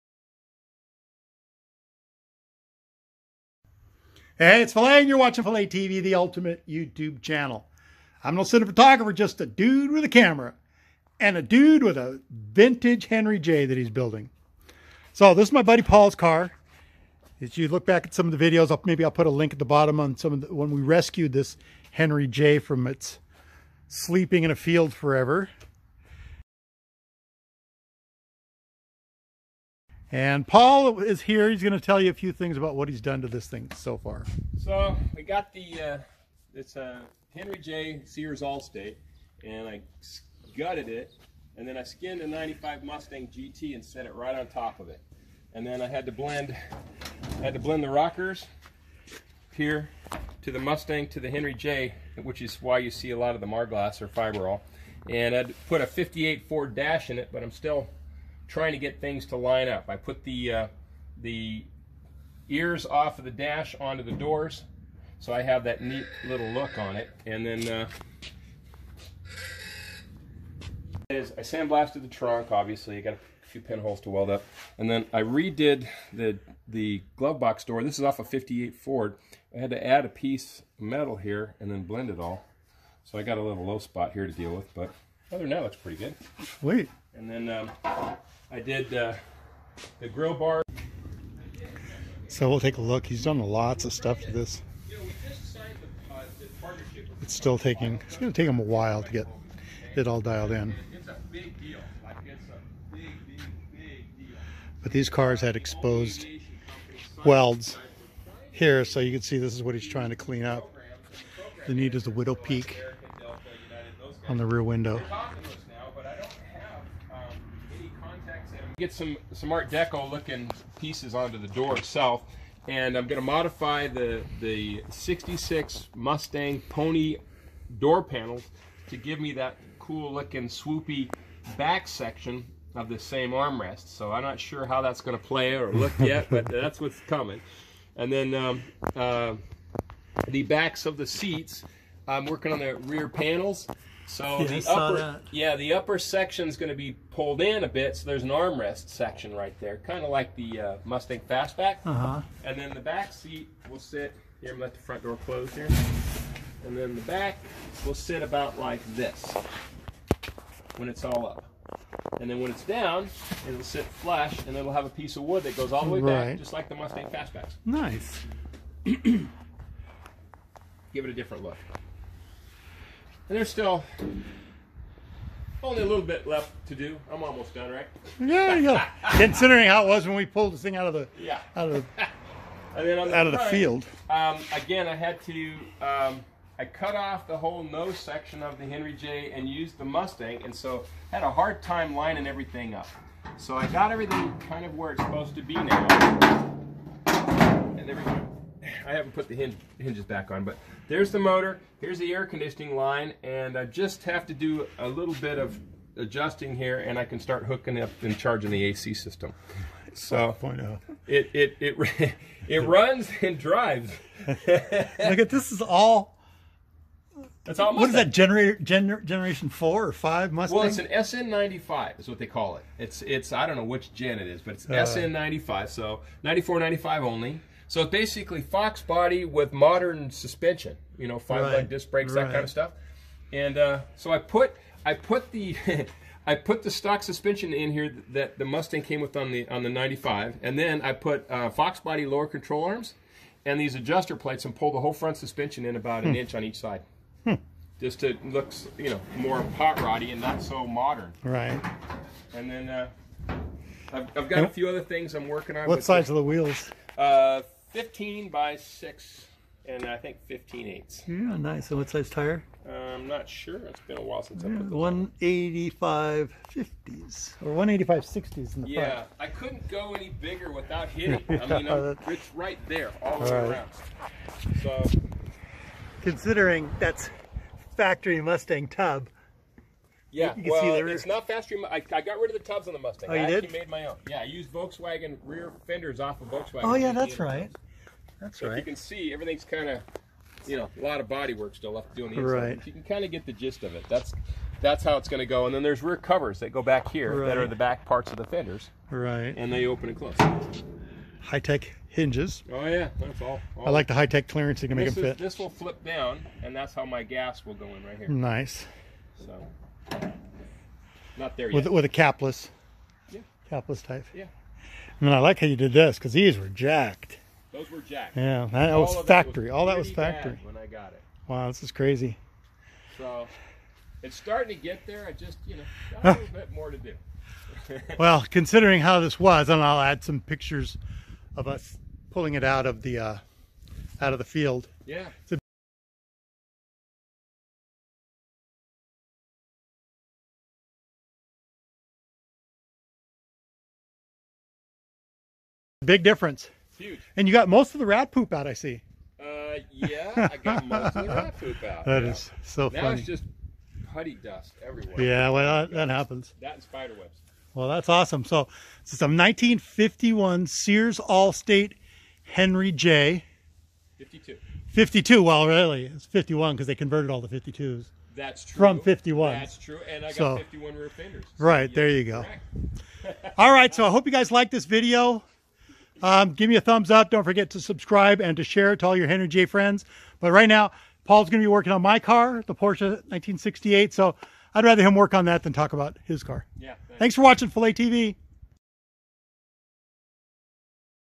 1952 HENRY J WITH A 5.0 MUSTANG FOX BODY RUNNING GEAR STREET ROD! PART 2 PHILA TV!
1952 HENRY J WITH A  5.0 MUSTANG FOX BODY RUNNING GEAR STREET ROD!  PART 2  PHILA TV!